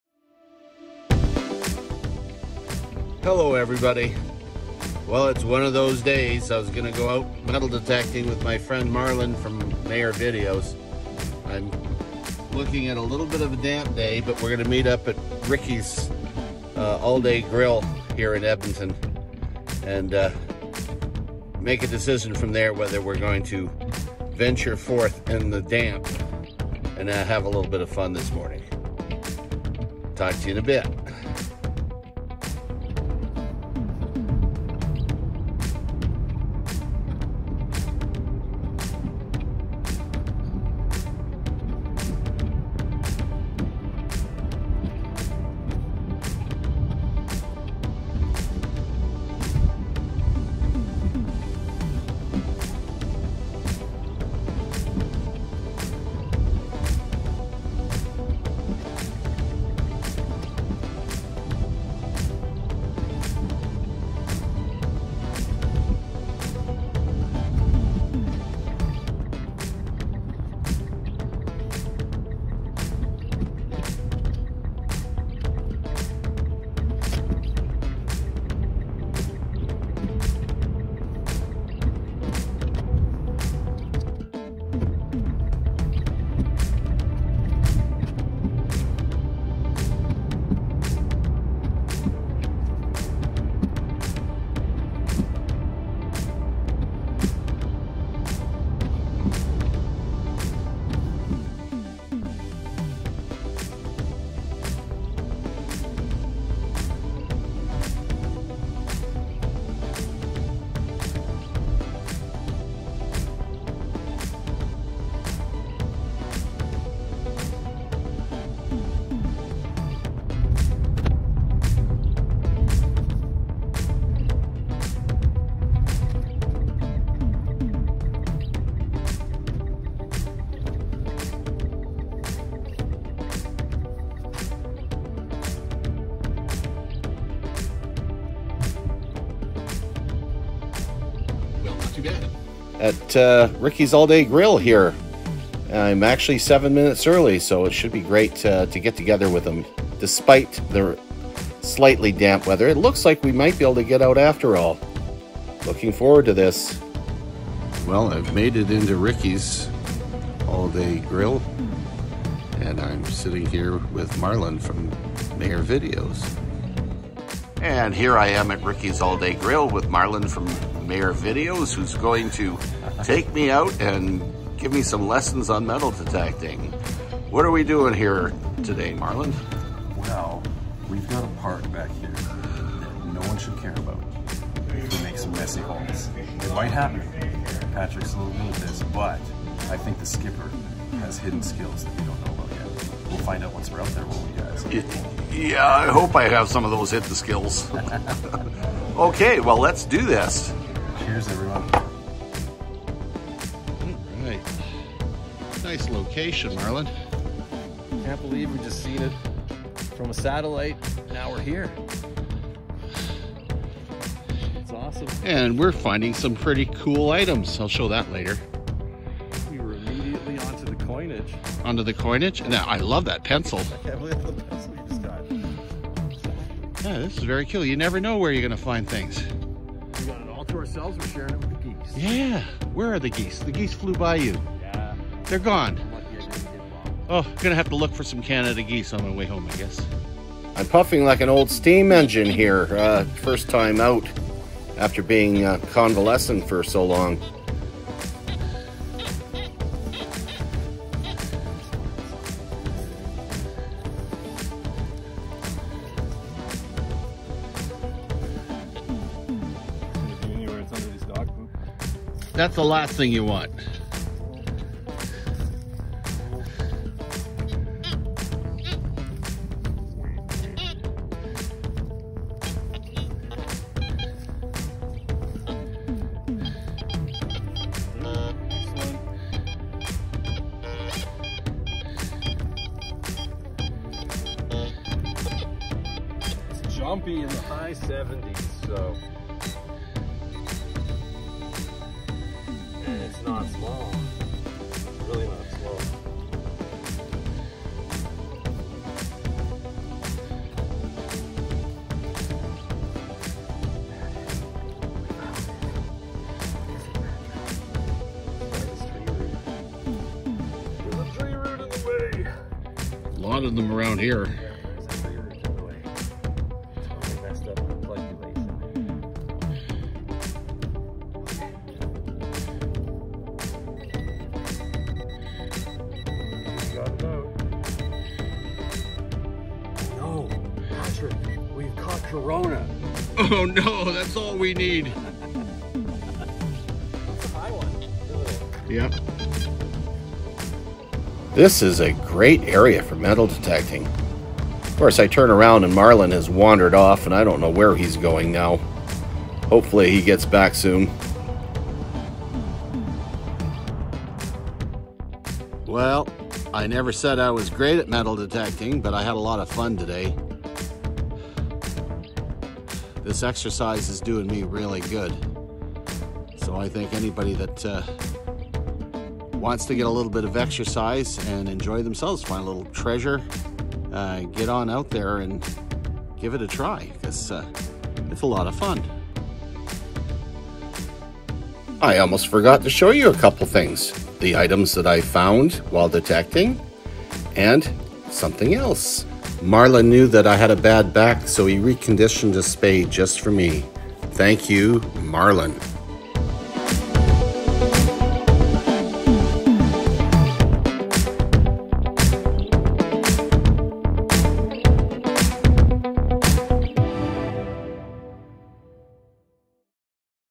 Metal Detecting with Maer Videos
My first time out with someone who actually knows what he’s doing! I had so much fun doing this and looking forward to doing it again! Always looking for the treasures to add to my Coin collection￼! We’re located in and around Edmonton Alberta Canada but plan on doing adventures throughout the province in the near future! The overlap between a Coin Collecting and metal detecting is quite significant! It’s amazing how many old pieces you can find to add to your collection! This is also great for those who need activities in order to improve health! I’m certainly going to enjoy doing it and I hope you do too!￼